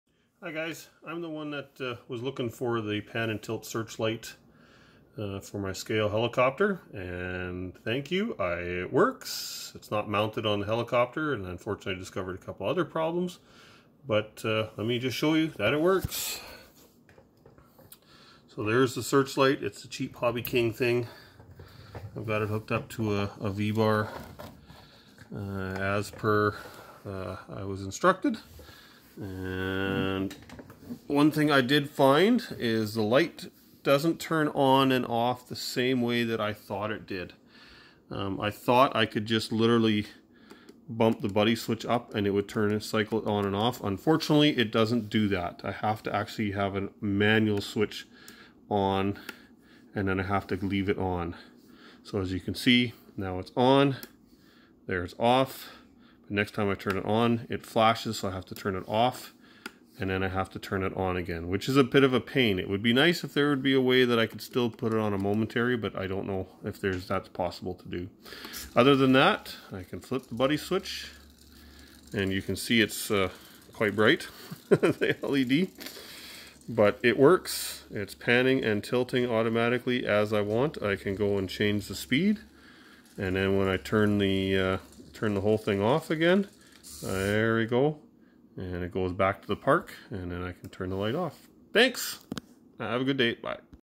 VBar Neo pan and tilt works
This is just in response to Rainer from Mikado and his help in getting the automatic pan and tilt, with a park function, working with my VBar Neo and VControl touch transmitter.  As you can see, it's not perfect but it will work well.  I may play around with what switch I use and the speed of everything but that's just minor tweaking and tuning.  Thanks Ranier.